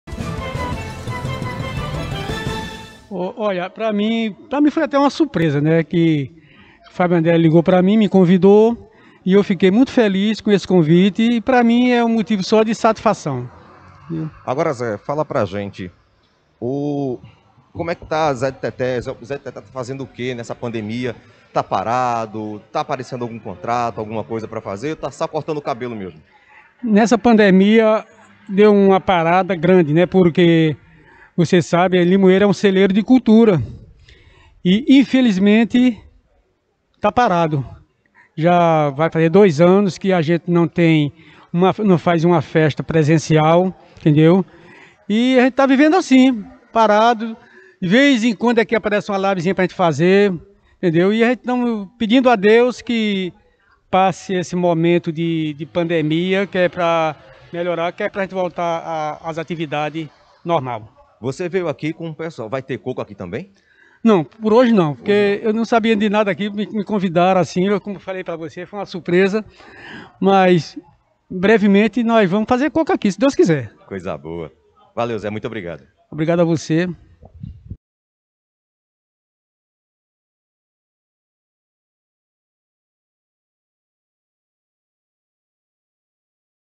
Zé de Teté - Ala dos Mestres é inaugurada no Oitão do Centro de Criação Galpão das Artes em Limoeiro
Nesta última terça-feira (13/07) aconteceu a inauguração da segunda etapa do Minimuseu Dona Daluz: a ala dos mestres, no Centro de Criação Galpão das Artes, Limoeiro - PE (agreste setentrional). Foram homenageados artistas, como Zé de Teté (coquista), Sinvaldo - Bolinha ( artesão da madeira), Rosélis Alves ( artista plástica), Jorge Raimundo (artesão do Mané Gostoso), Emanuel Sacramento (escultor), Salomé Barros ( artesã) e Maria José de Barros (filhas de D. Daluz).

No evento também o Coletivo Fulô da Mata (Joseane Rocha, Joice Andrade e Maria José de Barros)  exibiu o  curta "Miro, um sonho que virou boneco" e Rodrigo Sávio, historiador e membro da UCEA (União Carpinense de Escritores e Artistas) apresentou sua obra "Mestre Solon e o Mamulengo Invenção Brasileira". Um evento lindo que contou com a presença de várias autoridades da área da cultura como José  Manoel Sobrinho  (presidente da Fundação de Cultura da cidade do Recife) e a mestra, escritora e poetisa, Flávia Suassuna. Além da presença do produtor cultural Afonso Oliveira, responsável pelo método e Festival Canavial .